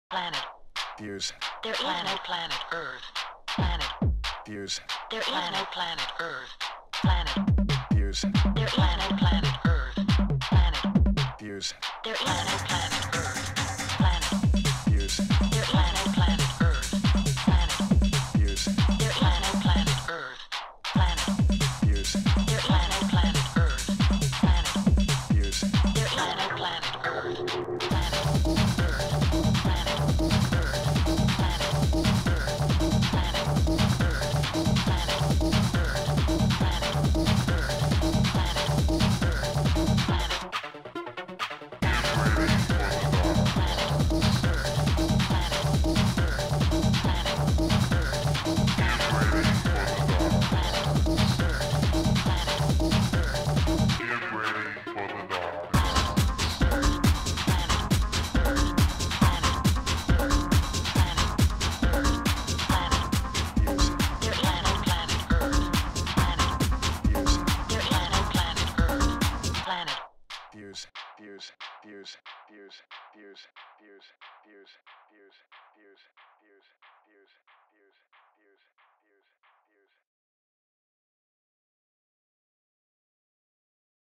Next Fuse Replay with Claude Young & the Advent on Tue. 30th April
Aftermovie Fuse Replay with Laurent Garnier and trailer :-) of Fuse Replay with Claude Young and the Advent on Tuesday 30th April at Fuse.  Check for more info!